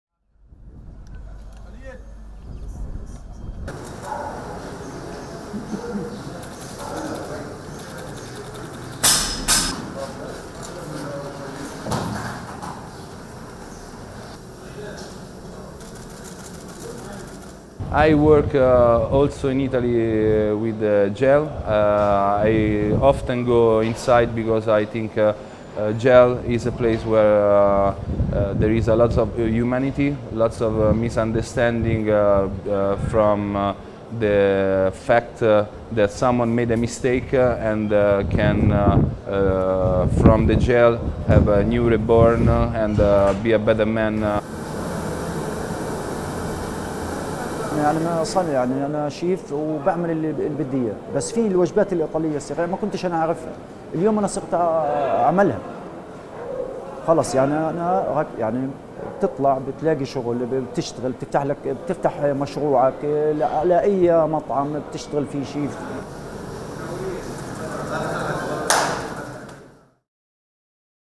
Un chef italien enseigne l'art des pâtes... à des prisonniers de Gaza | AFP News
Partager ses astuces pour cuisiner des pâtes parfaites, il n'y a rien de plus ordinaire pour Gabriele Rubini. Mais cette semaine, ce chef italien a innové: c'est à des prisonniers de la bande de Gaza qu'il a donné des cours de cuisine.